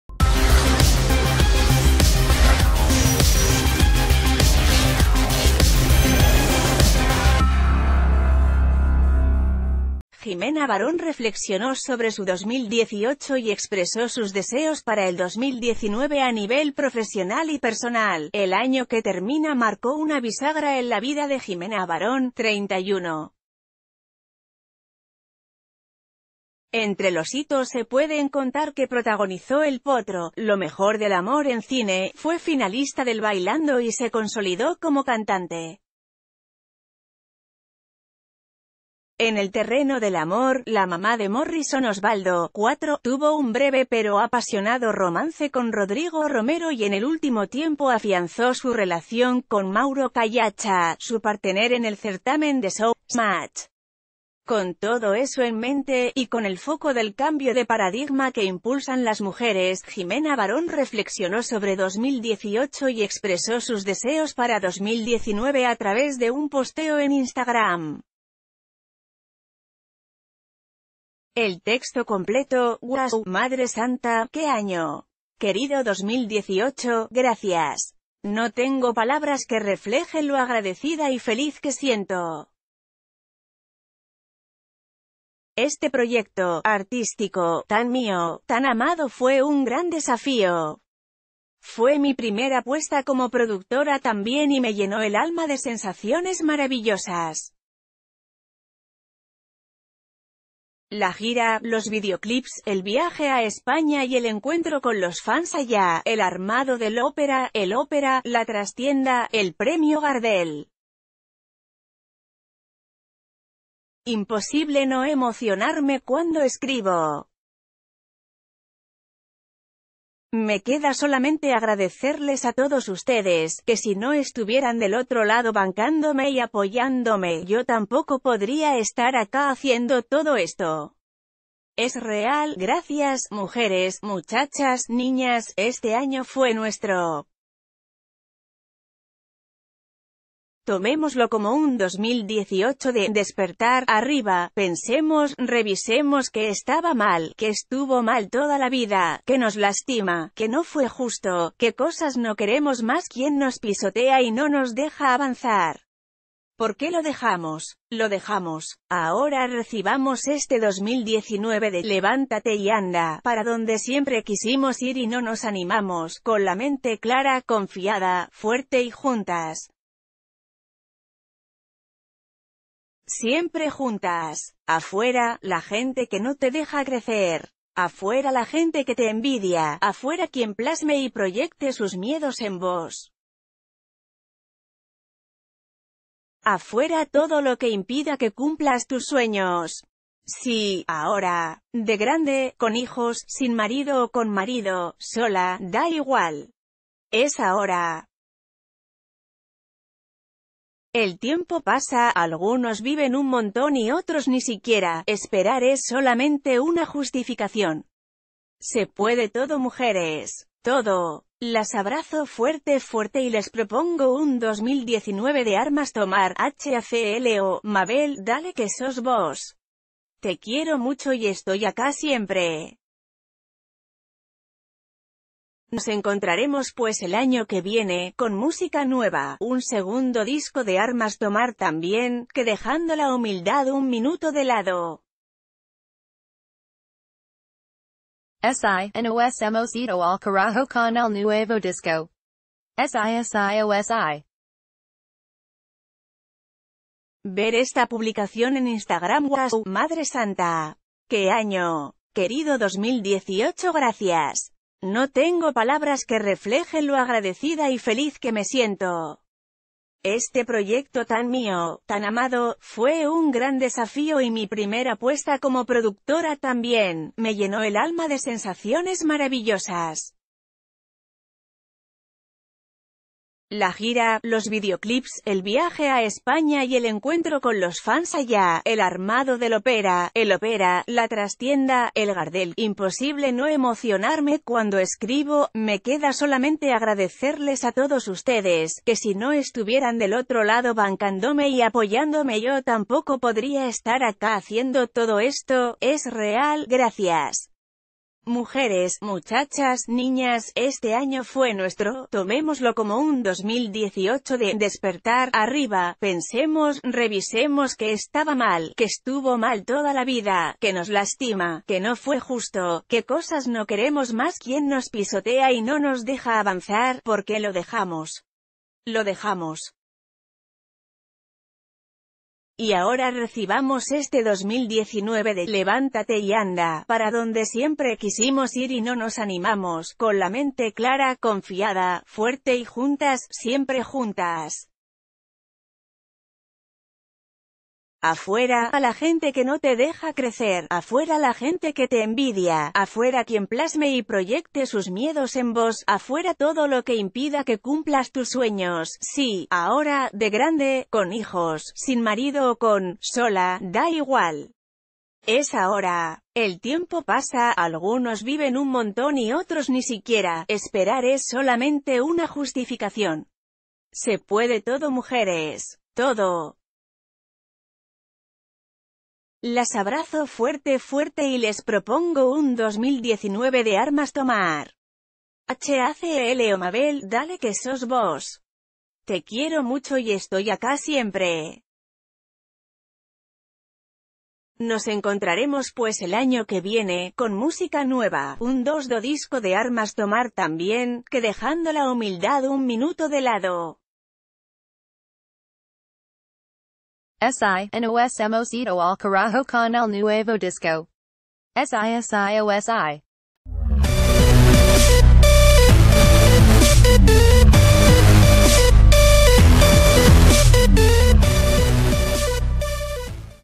Jimena Barón, reflexiva: el balance sobre su 2018 y sus deseos para 2019
Jimena Barón reflexionó sobre su 2018 y expresó sus deseos para el 2019 A nivel profesional y personal, el año que termina marcó una bisagra en la vida de Jimena Barón (31). Entre los hitos se pueden contar que protagonizó El Potro, lo mejor del amor en cine, fue finalista del Bailando y se consolid...